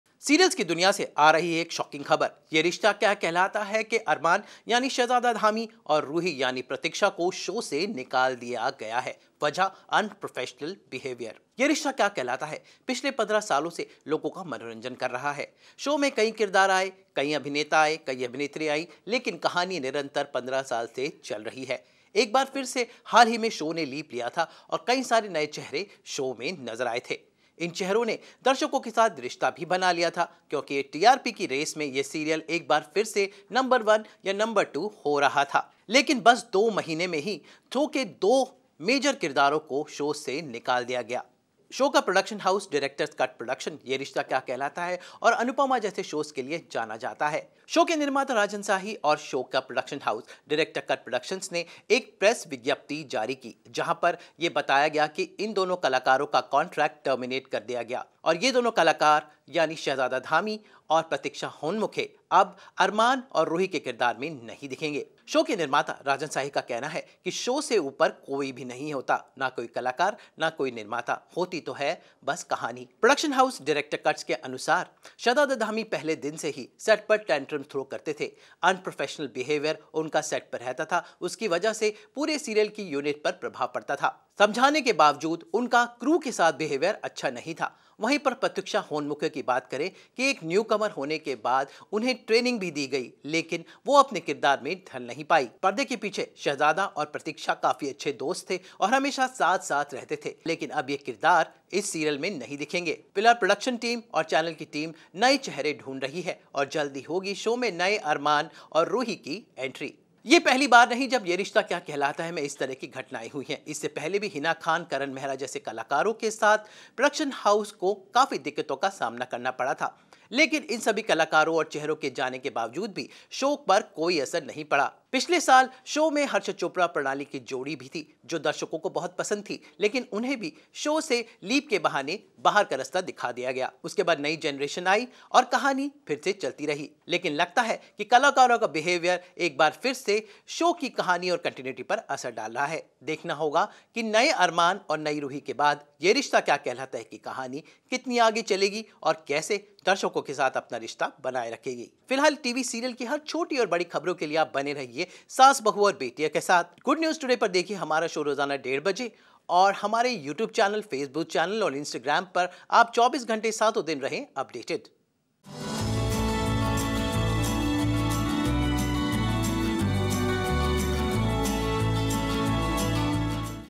Why Shehzada Dhami and Pratiksha Honmukhe Got Replaced From Yeh Rishta Kya Kehlata Hai? | SBB
Reasons Explained In Video